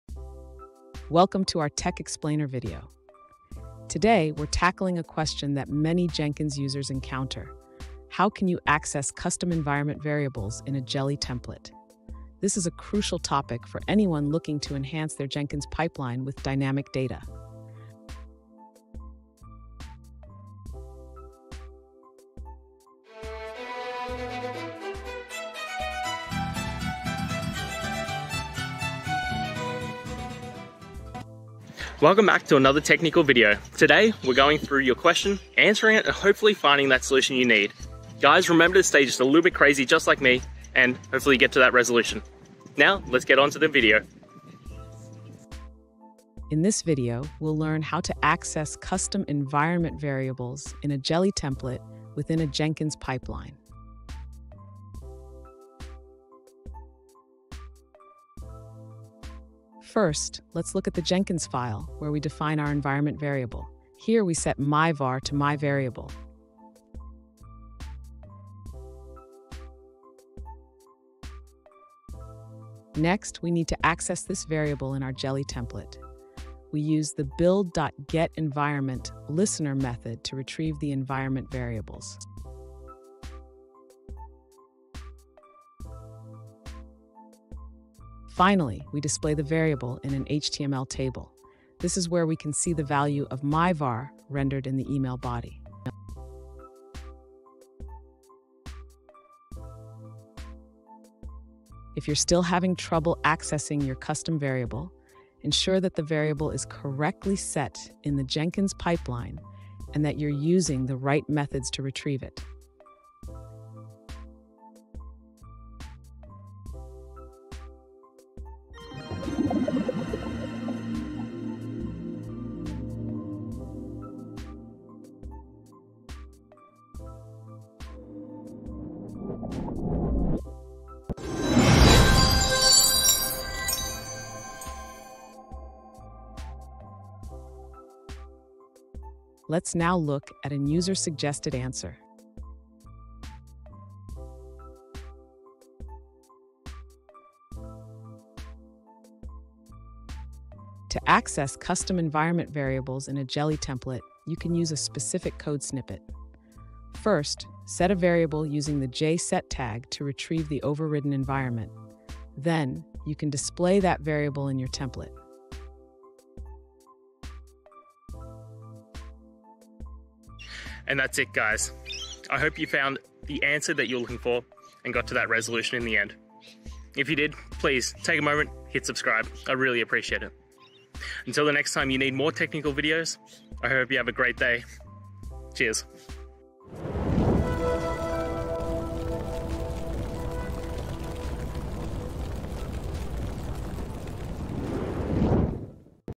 How to Access Custom Environment Variables in Jelly Templates
In this video, we’ll explore the powerful capabilities of Jelly templates and how to leverage custom environment variables to enhance your projects. Whether you're looking to streamline your development process or personalize your templates, understanding how to access and utilize these variables is essential. Join us as we break down the steps and provide practical examples to help you get the most out of your Jelly templates.

Today's Topic: How to Access Custom Environment Variables in Jelly Templates

Related to